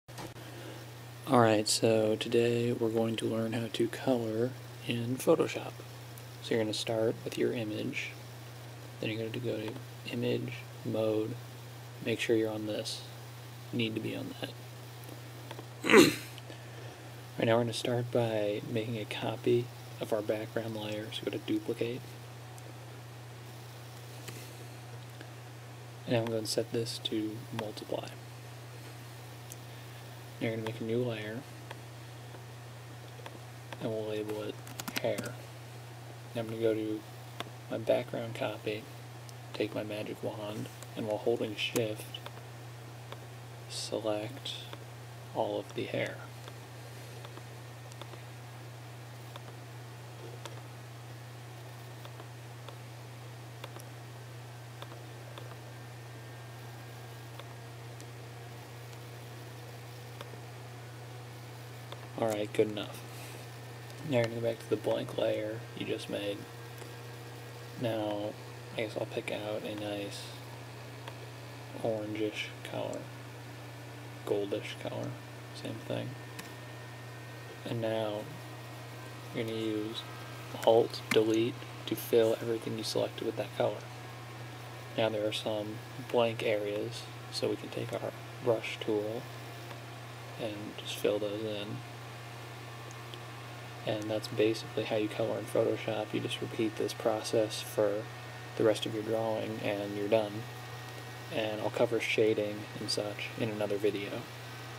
How To Color A Drawing in Photoshop
A friend of mine asked me a question, "how do I color in Photoshop?" This video is made to answer his question. This was done using Photoshop CS4 but should work with any other version of Photoshop. I will cover shading in another video.